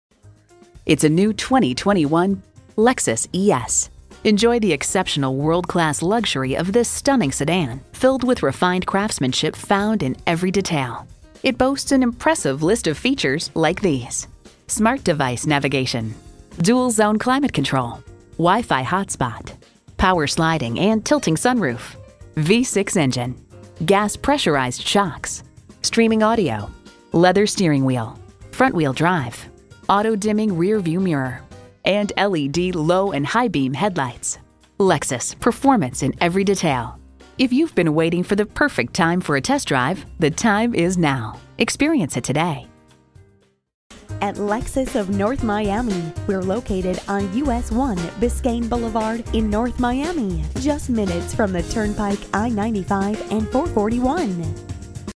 New 2021 Lexus ES Miami, FL #21103500
Year: 2021
Make: Lexus
Model: ES
Trim: 350
Engine: 3.5L V6 DOHC 24V
Transmission: 8-Speed Automatic
Color: Black
Interior: Chateau Perforated Nuluxe
Stock #: 21103500
VIN: 58ACZ1B10MU086170
Caviar 2021 Lexus ES 350 FWD 8-Speed Automatic 3.5L V6 DOHC 24V Chateau Perforated Nuluxe.brbrRecent Arrival! 22/32 City/Highway MPG

Address:
14100 Biscayne Blvd